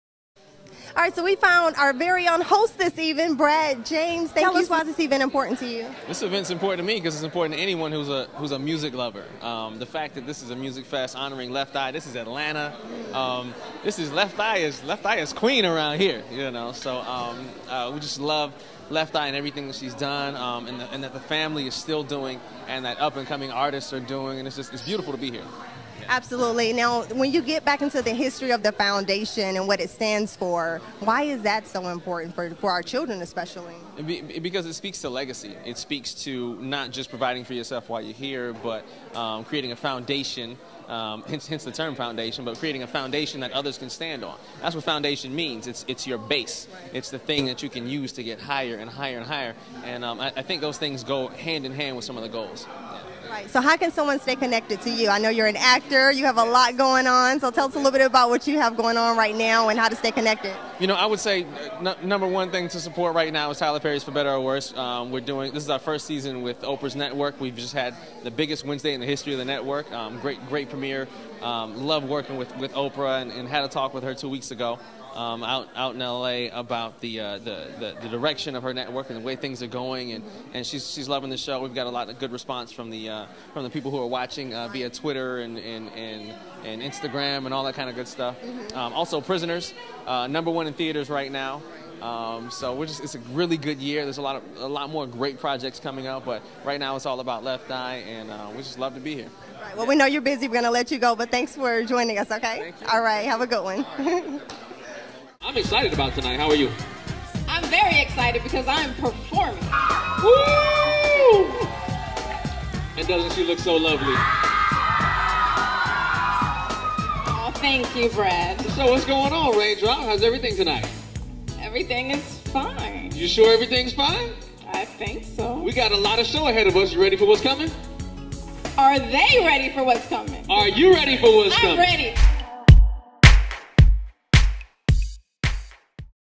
GlenNeta Griffin Interviews Tyler Perry Star, Actor | Host Brad James
Left Eye Music Fest
Follow @glennetagriffin